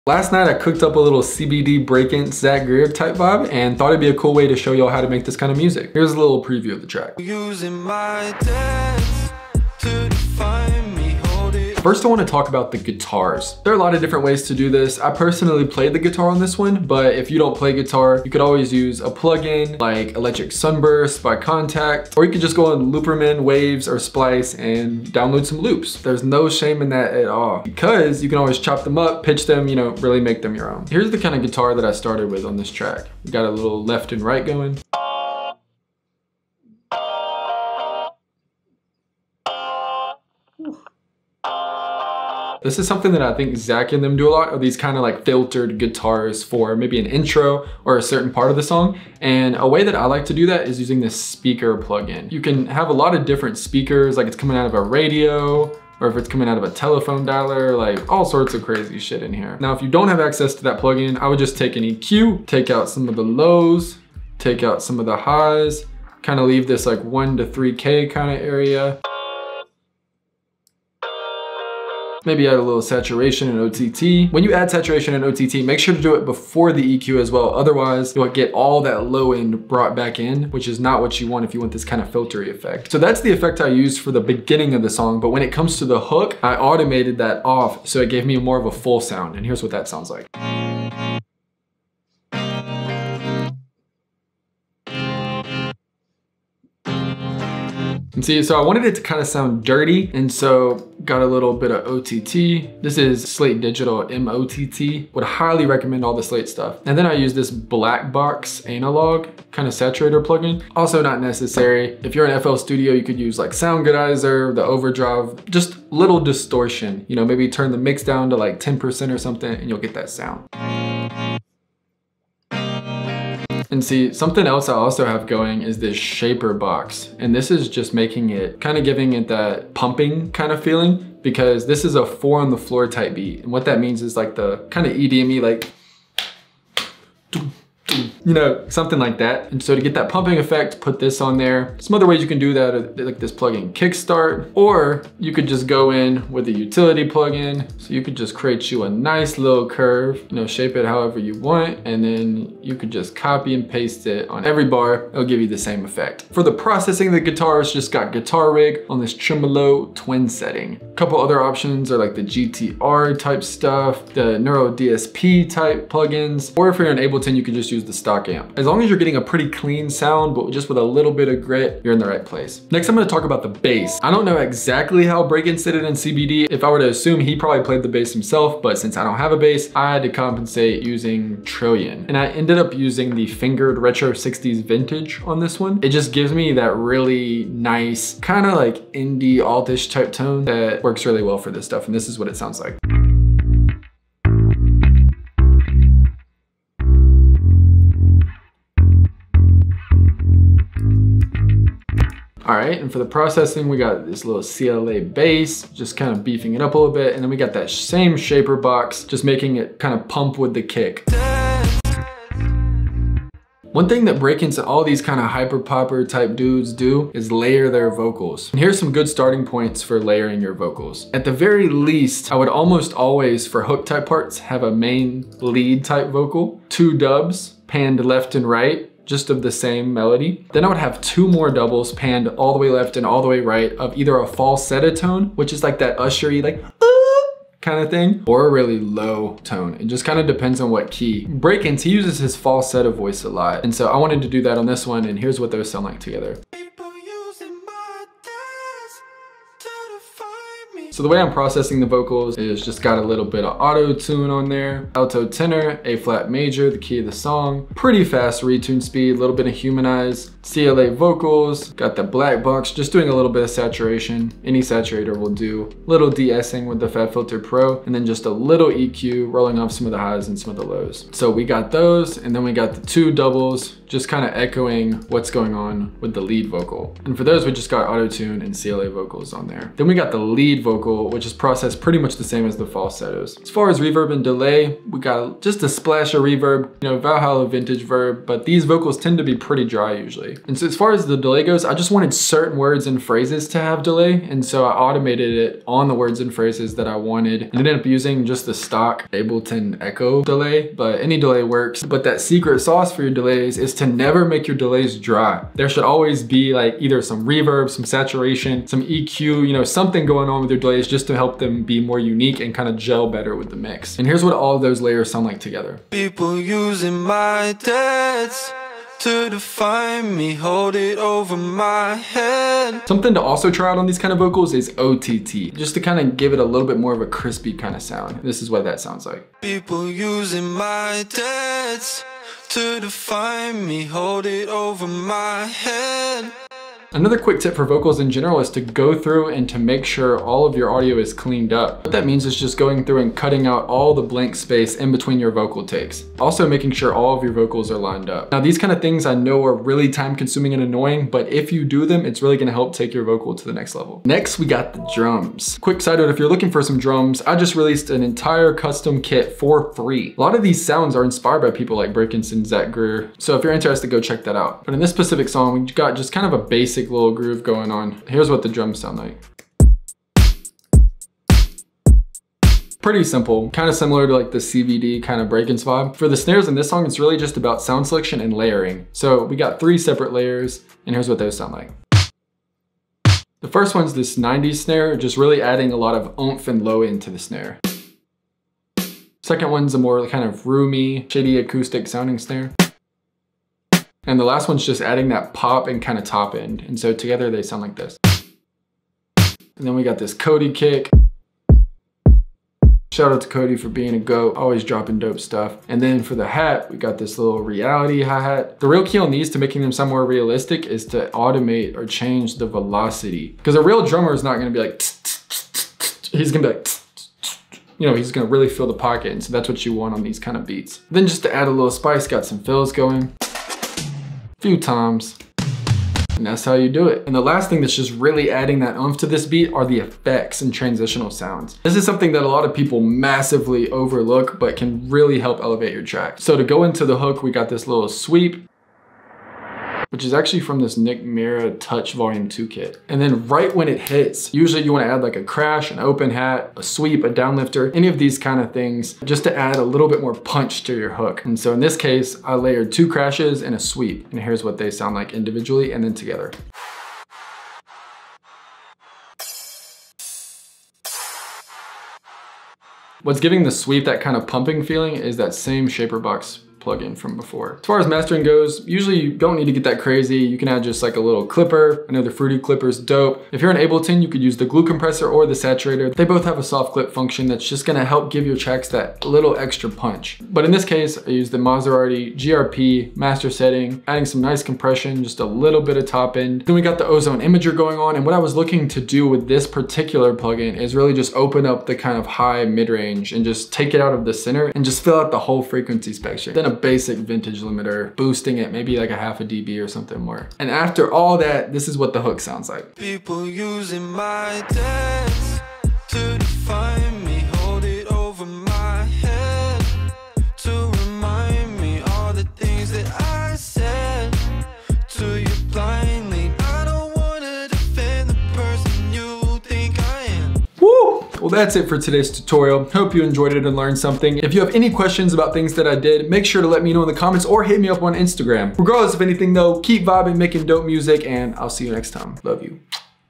How to Make a Brakence CBD/Zac Greer Type Song!
This is a tutorial on how to make a song like Brakence and Zac Greer!
Made this vibe a couple days ago and figured I would share!